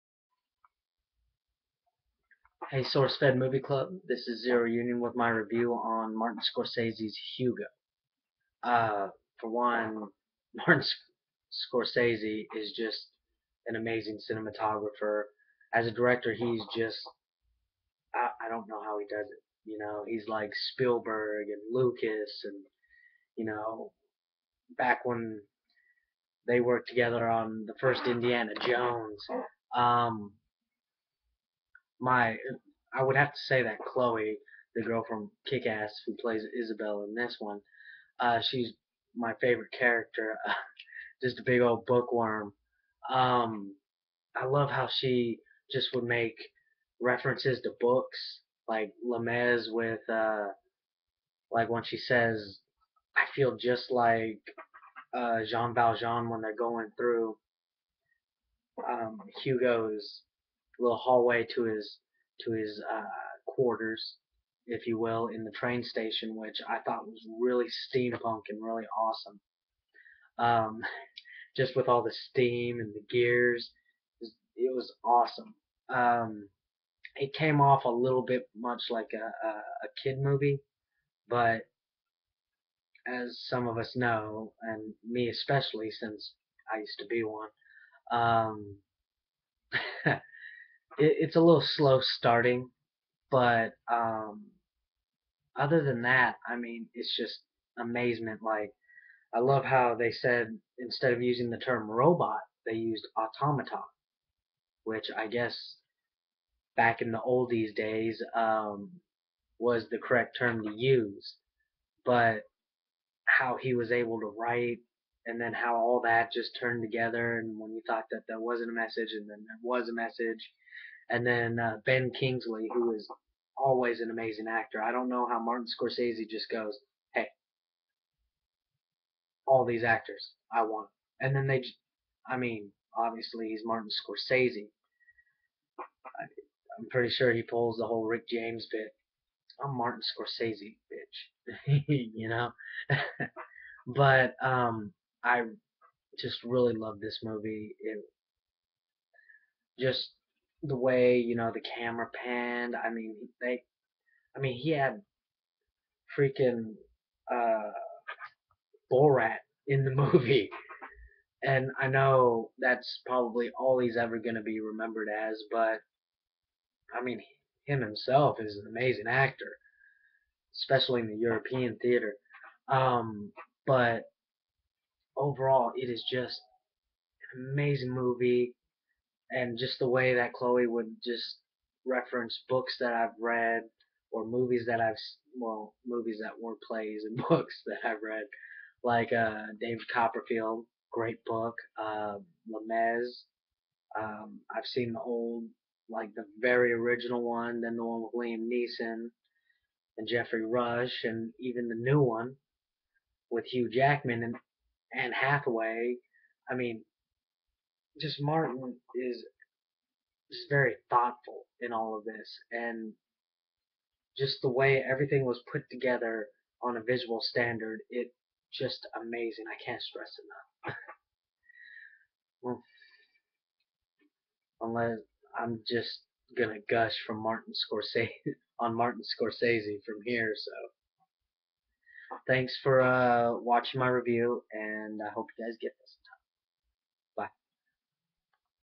sourcefed movie club:Hugo (zer0union)
this is my opinion on how great this movie was